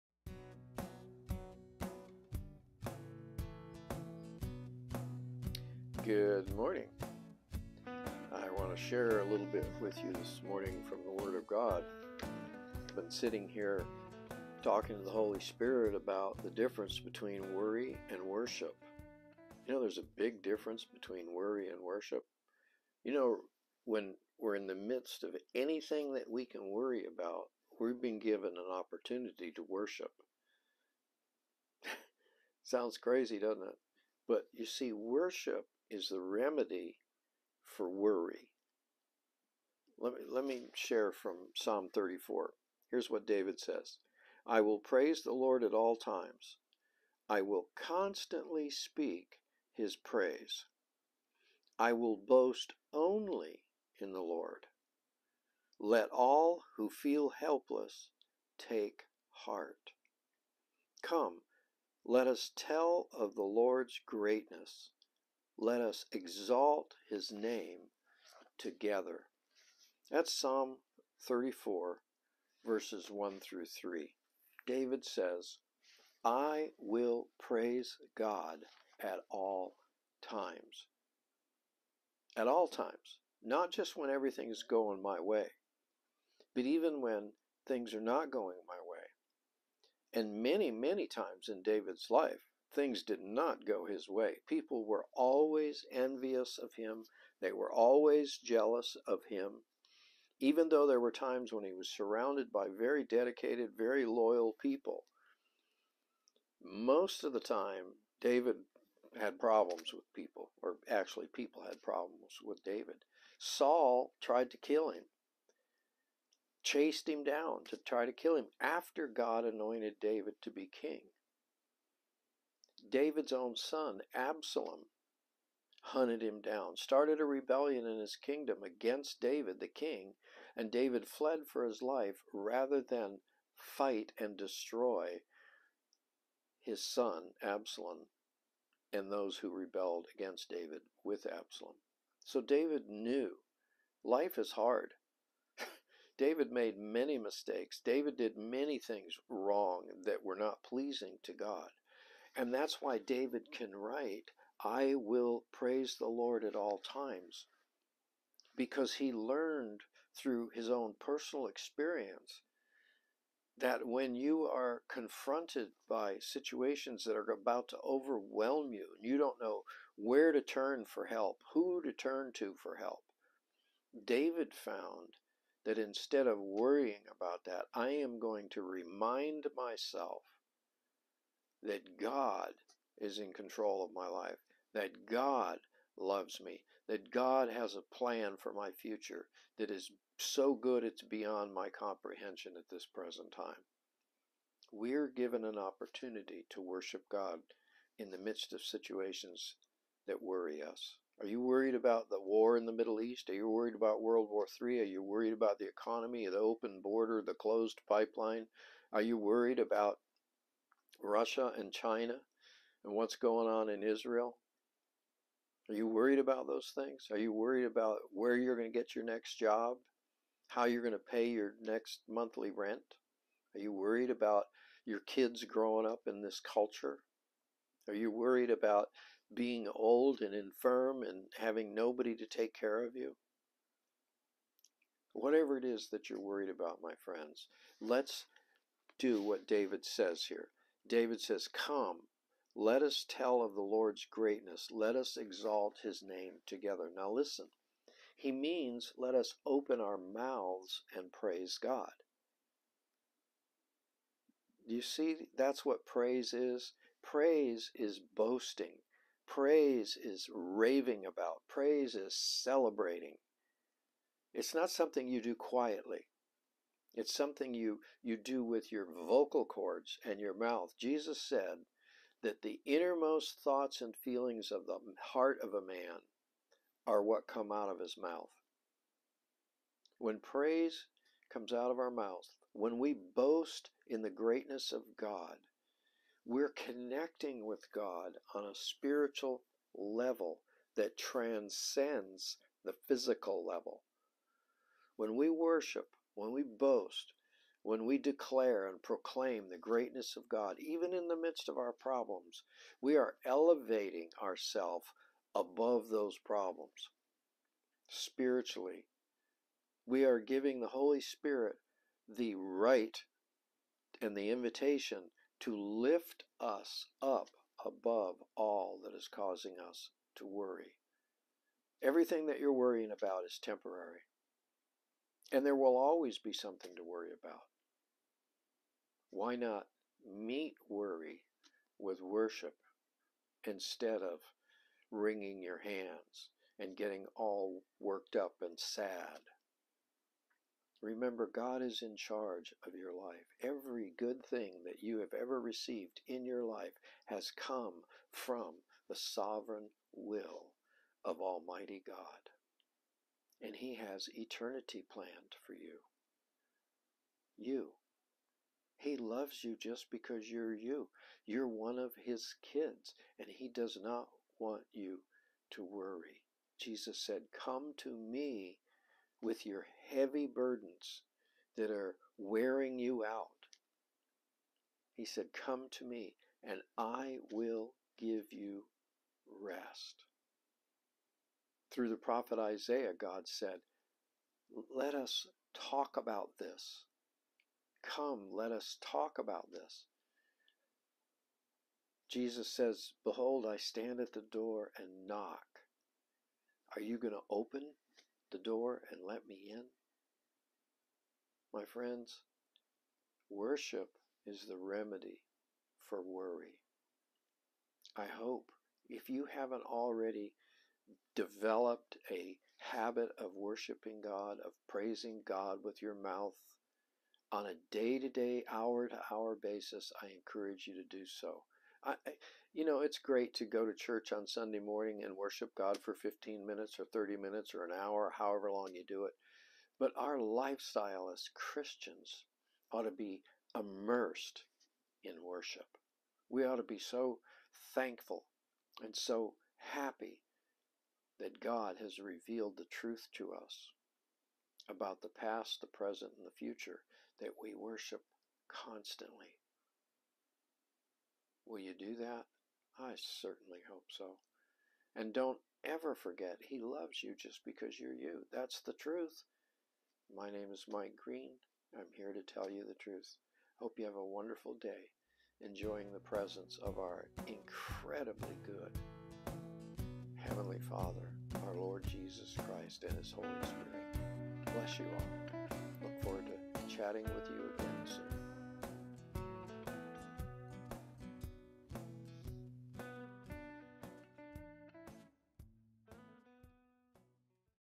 Worry or Worship
There's a bunch of stuff to worry about today! War, rumors of war, the economy, the rise of antichrist, and liars, deceivers, and cheaters are everywhere! To whom do I turn for relief? GOD is available my friends! GOD! He is always everywhere present to help. Turn to Him and worship Him! Open your mouth and praise Him, boast of His goodness, rave about His love!